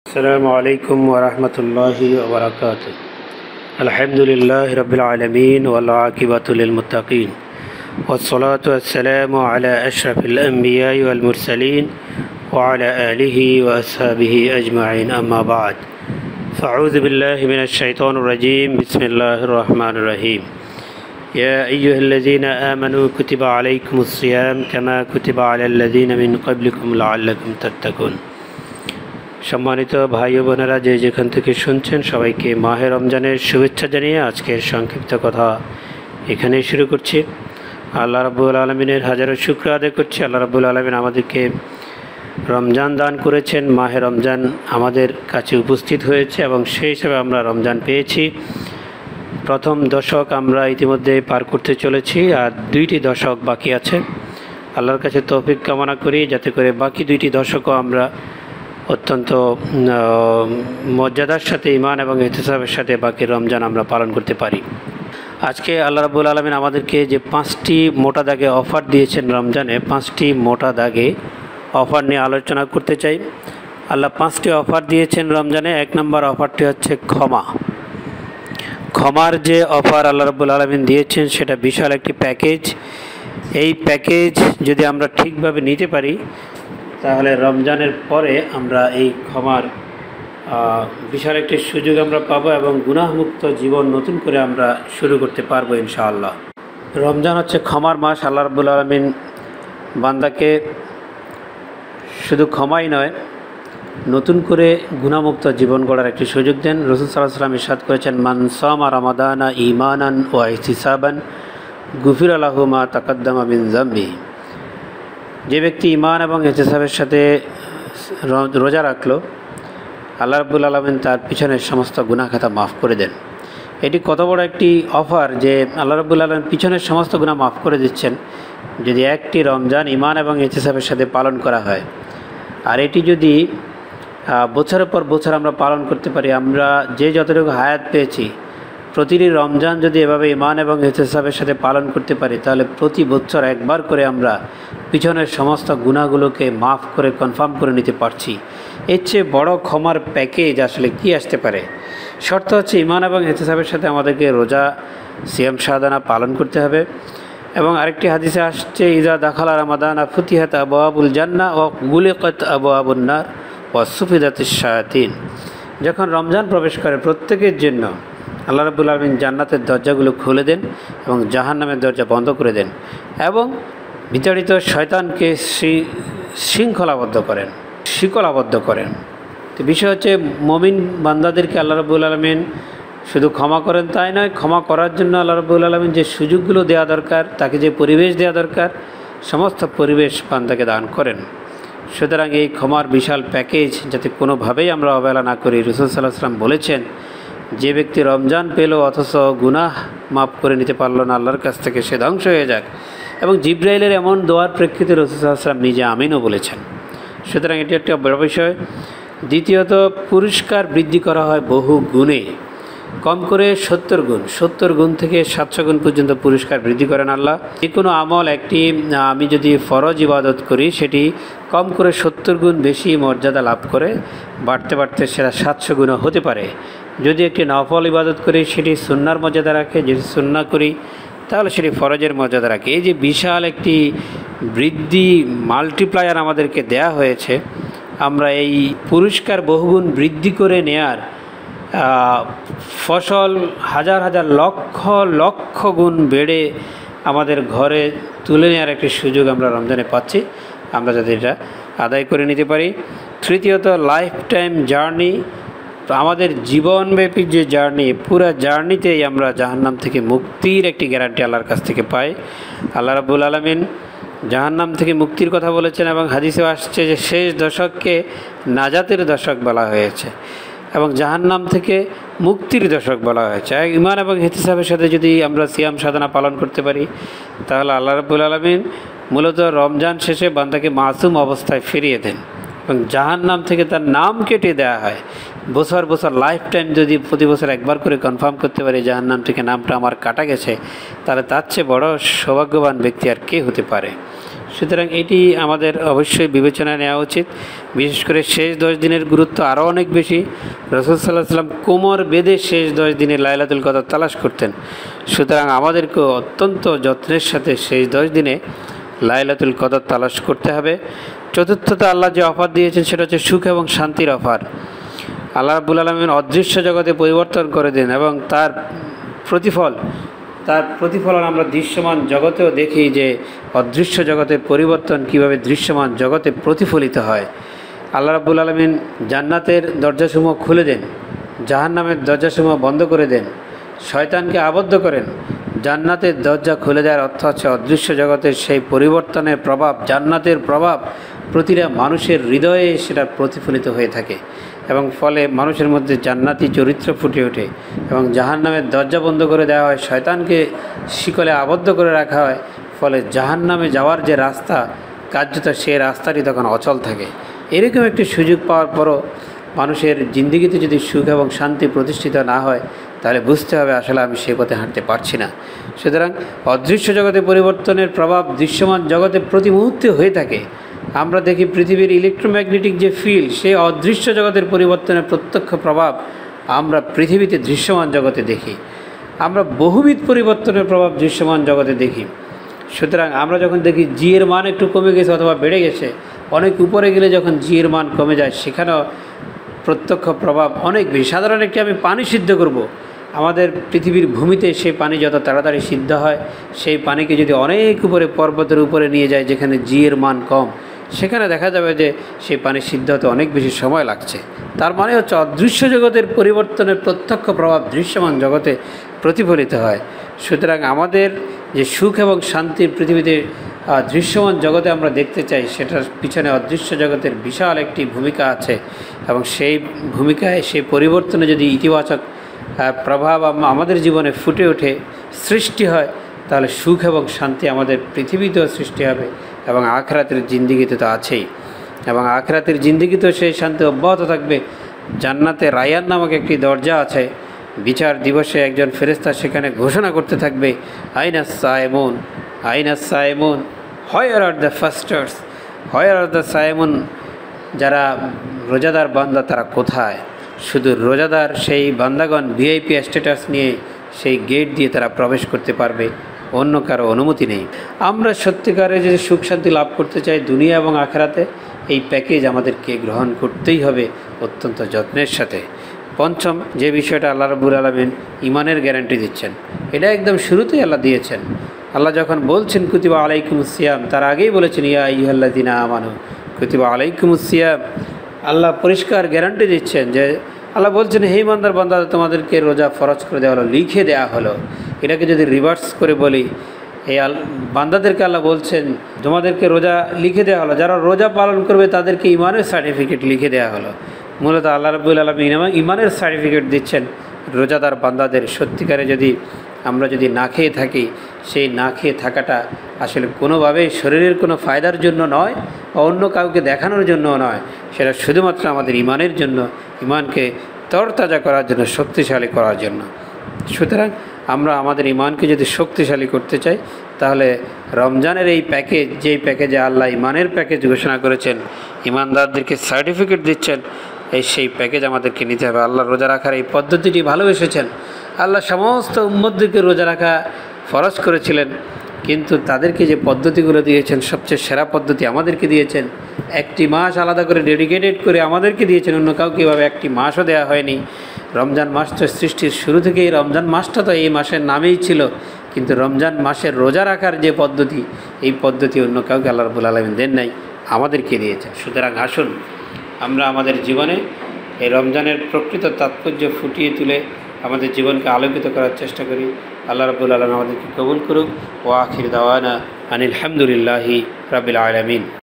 রমযানে আল্লাহর ৫টি অফার
ক্ষমা
পুরস্কার বহুগুন বৃদ্ধি
জীবনব্যাপী জাহান্নাম থেকে মুক্তি
সূখ- শান্তি
ঈমানের গ্যারান্টি
Dr. Mostafizur Rahman Faisal